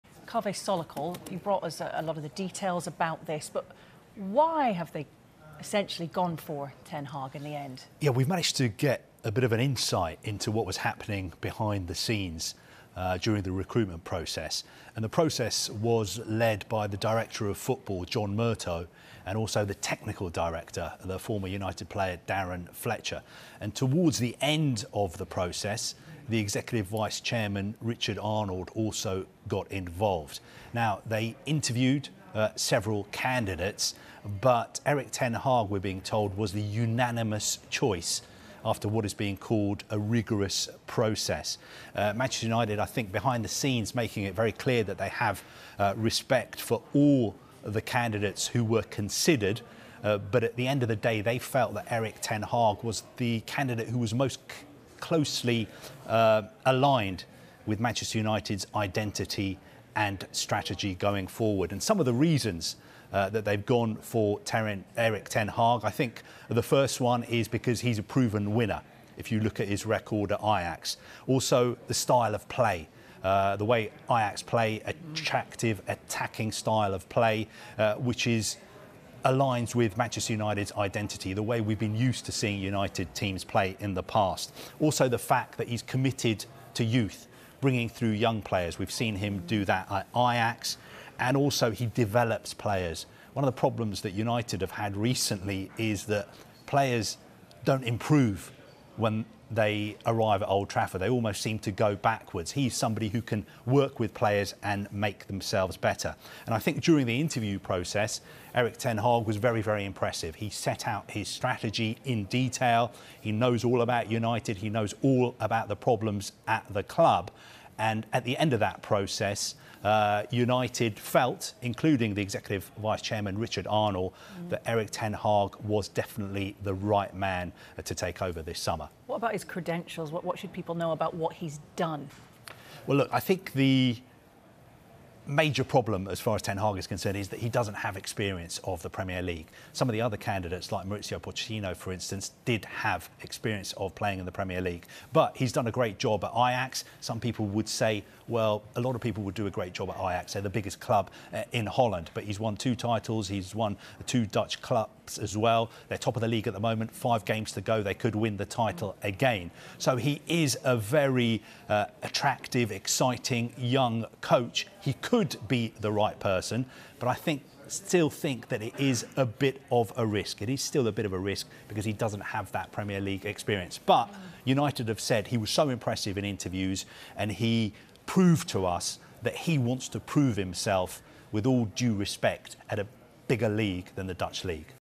Is Erik Ten Hag a risk for Manchester United? 🤔 | Sky Sports chief reporter Kaveh Solhekol reacts
Sky Sports chief reporter Kaveh Solhekol explains why Erik ten Hag has been hired as Manchester United manager and why his hiring might be a risk.